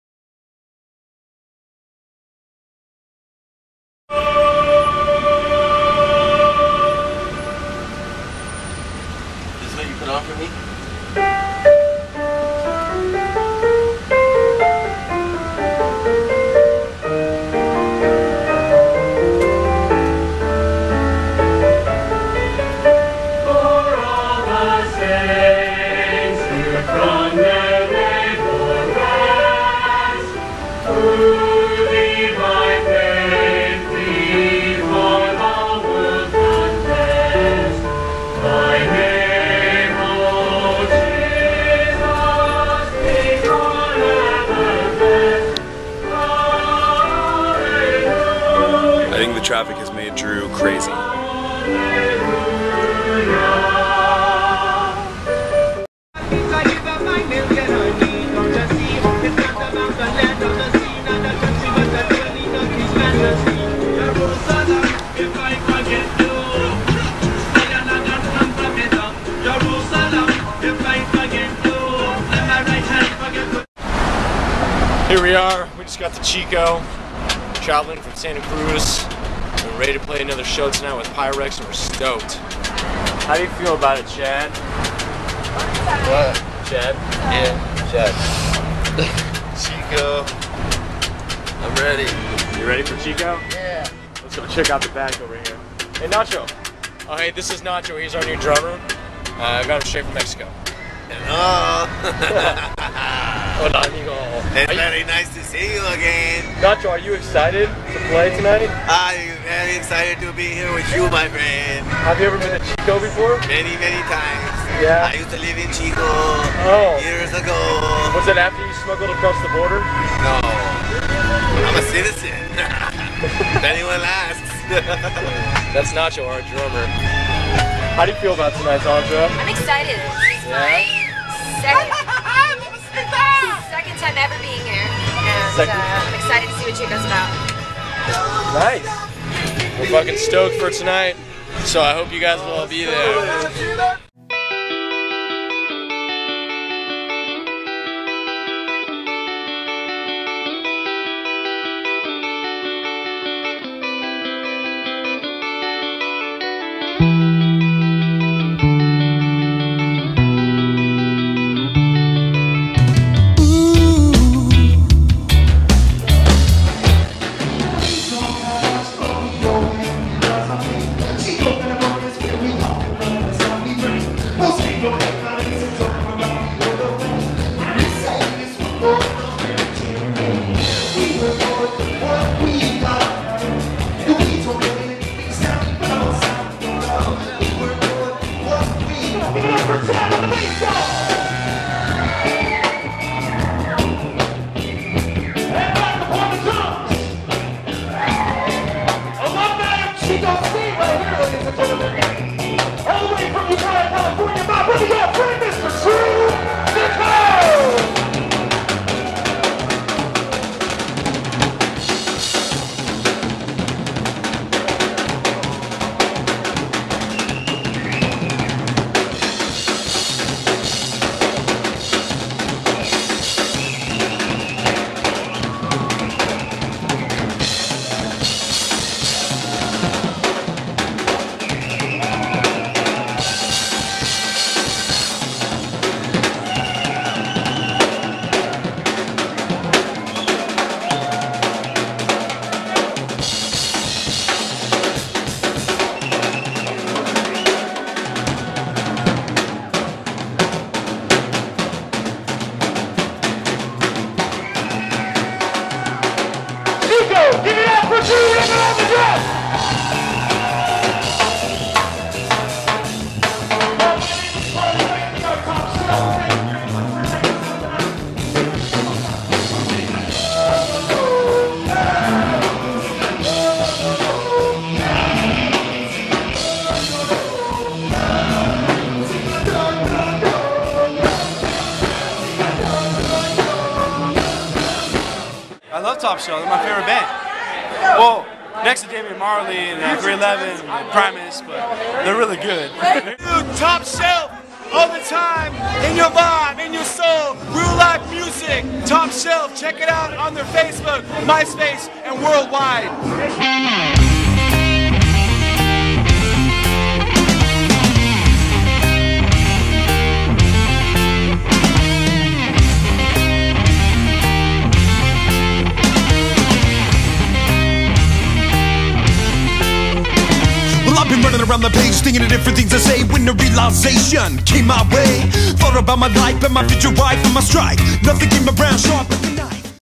Top Shelf in Chico @ Lost On Main
Top Shelf's spring tour takes them back to where they started 7 years ago...